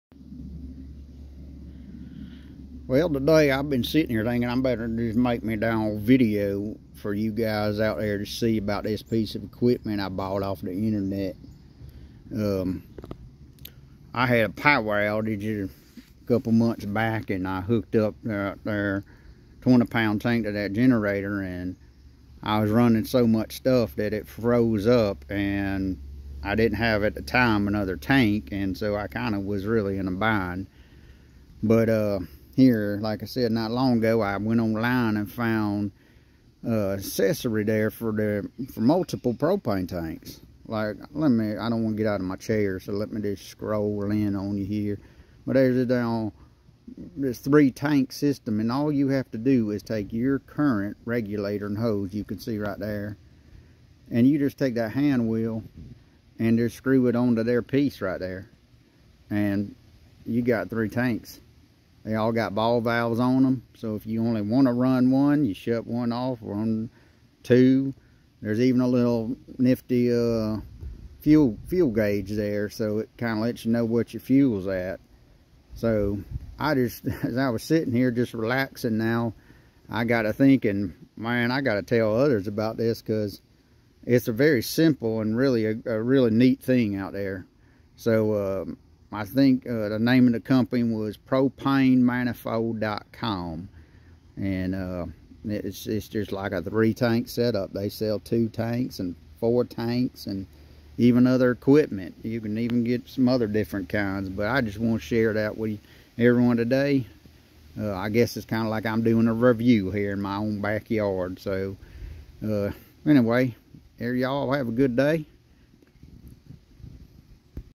Propane Manifold Thingamajigs
Well, hey there, y'all! Gather 'round 'cause I got somethin' real special to share with ya. Just got me this here brand-spankin' new propane manifold equipment off that fancy internet contraption, and boy, oh boy, lemme tell ya, it's a real game-changer!

Now, picture this: You're out in the woods, huntin' or campin' with your buddies, and ya need some serious firepower for cookin' up a feast fit for a king. Well, this here propane manifold is gonna be your best friend, I tell ya what.

See, it's got not one, not two, but a whole buncha connectors on it, so you can hook up all them propane tanks at once! Ain't that somethin'? No more fussin' 'round with one measly tank runnin' outta gas on ya. With this bad boy, you'll have enough propane power to fry a whole mess of catfish or grill up some juicy steaks to perfection.

And lemme tell ya, it's built tough as nails. Made outta sturdy metal that'll withstand any rough 'n tumble adventure you throw at it. Plus, it's got these nifty knobs for turning of the propane flow.

I reckon this propane manifold is gonna be a real hit at the next family reunion or tailgate party. Just imagine the envy on your cousin Cletus' face when he sees you effortlessly grillin' up them burgers while he's still strugglin' with his puny ol' grill.

So, if you're lookin' to up your outdoor cookin' game, head on over to that there internet thingy and type in and get yourself one of these propane manifold thingamajigs. Trust me, y'all won't regret it. Now, if you'll excuse me, I got some ribs to smoke and a cold one callin' my name. Yeehaw!